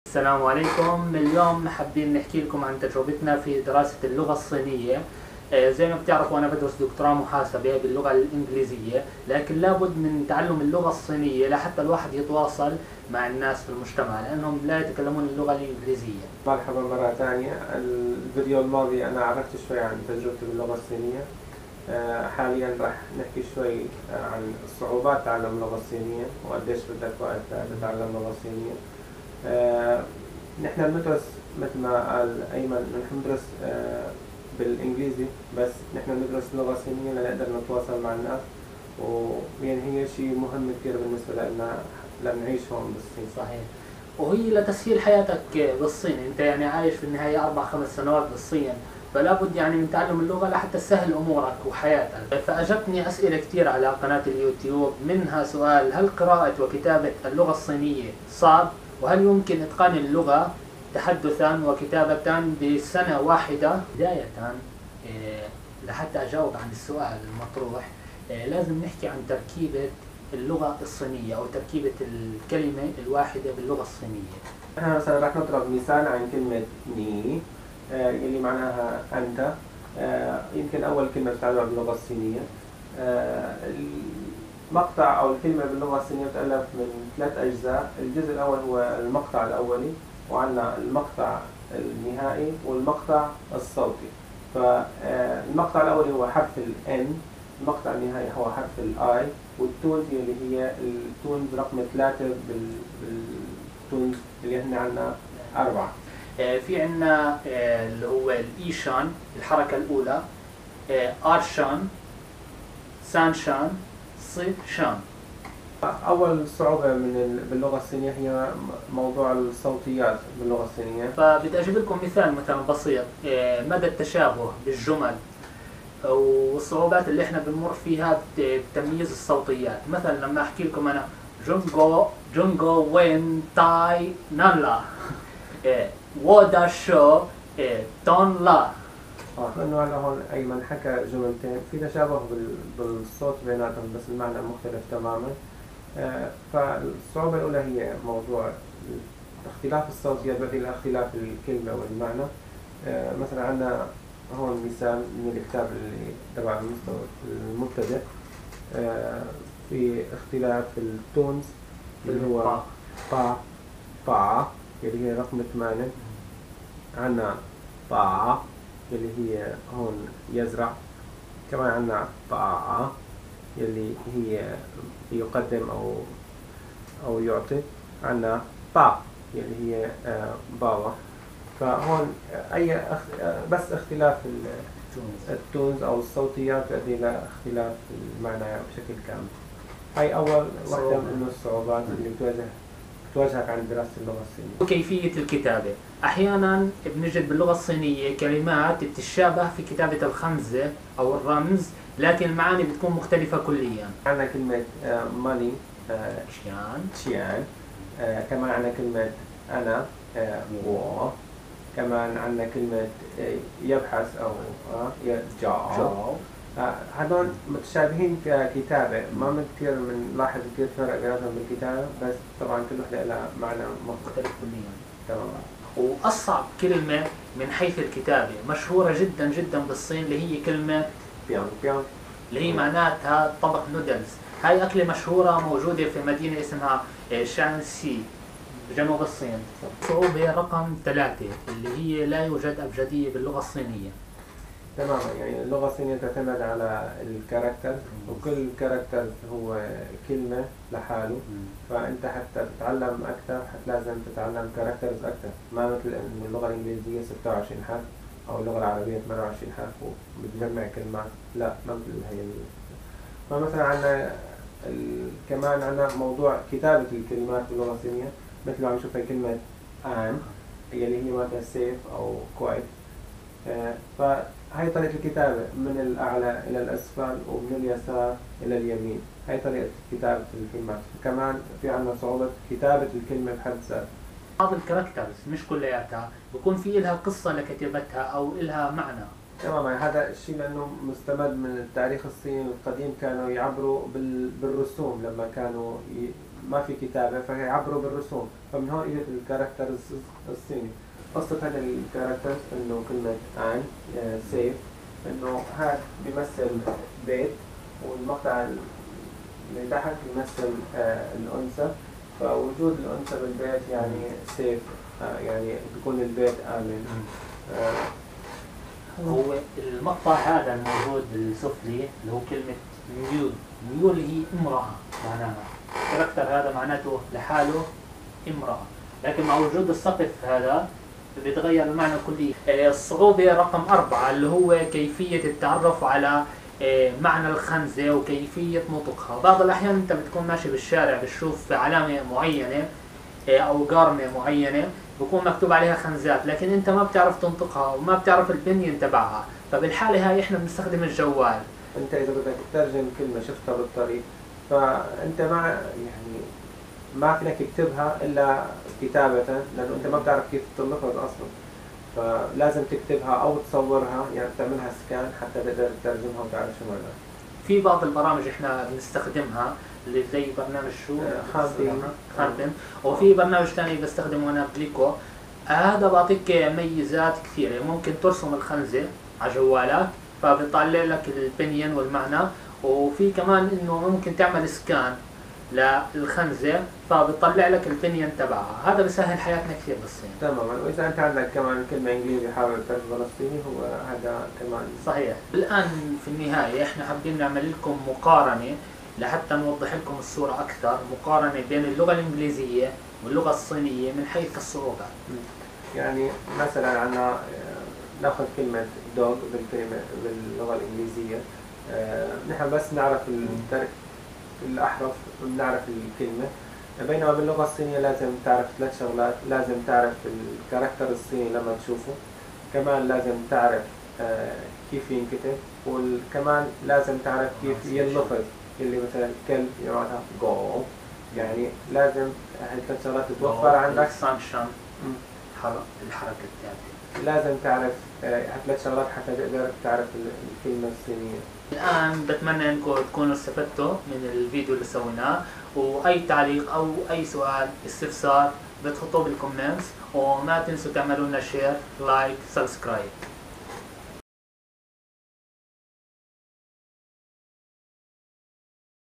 هل اللغة الصينية صعبة؟
تحدثنا عن الصعوبات التي تواجهنا في تعلم اللغة الصينية.